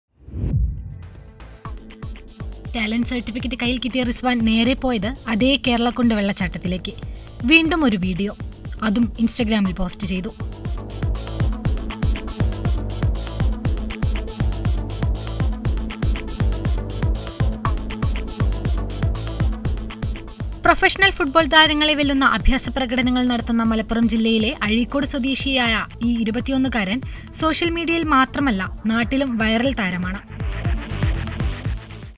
'മായമില്ല, മന്ത്രമില്ല'; റിസ്‌വാൻ കാല്‍പന്ത് മാന്ത്രികനാണ്, ഇൻസ്റ്റഗ്രാമില്‍ സൂപ്പർഹിറ്റും
അഴീക്കോട് സ്വദേശി റിസ്‌വാന്‍റെ കാലും പന്തും തമ്മില്‍ അത്രമാത്രം ആത്മബന്ധമുണ്ട്. അതു തന്നെയാണ് മാന്ത്രികതയ്ക്ക് പിന്നിലെ രഹസ്യവും